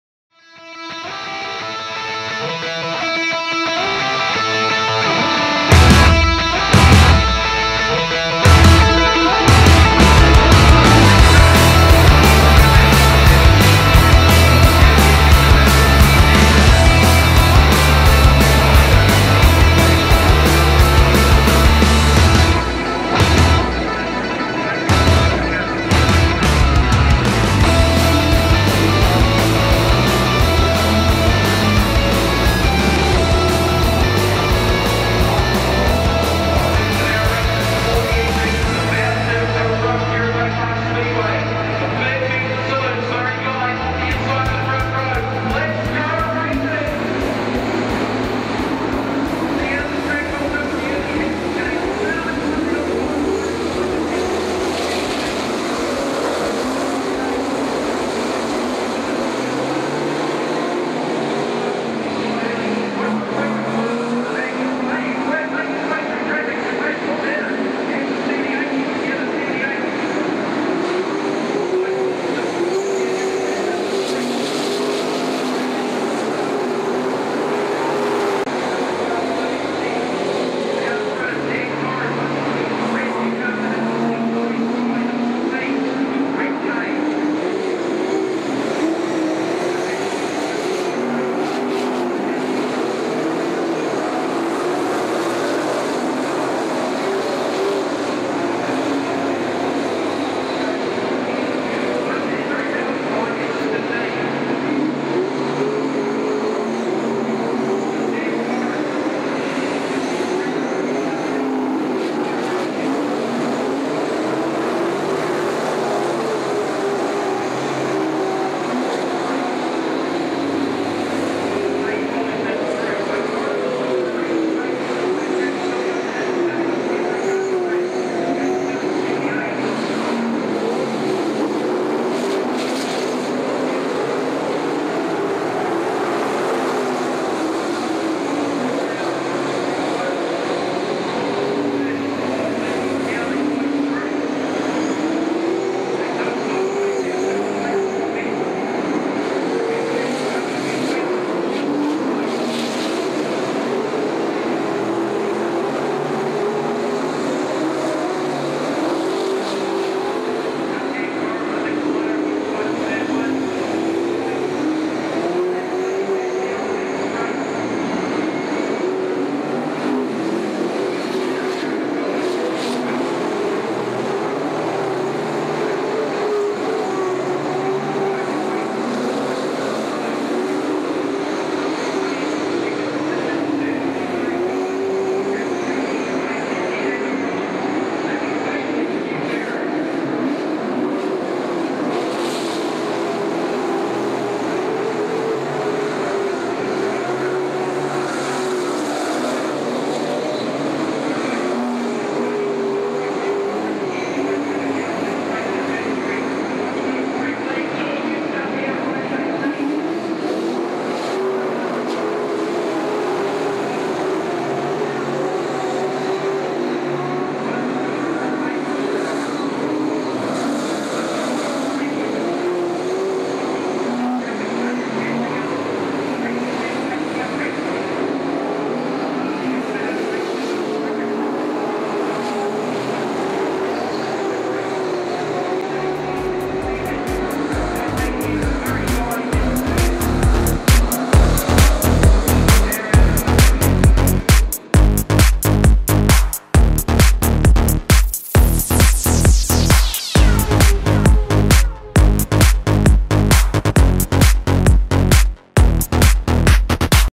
Baypark Speedway - Saloon Cars - Race01 - Meeting01 2011/12
Heat One of the Saloon Cars from Opening night at Baypark Speedway in Mt Maunganui.

Thanks to Zach Rayner and Dan Corrin for the In-Car footage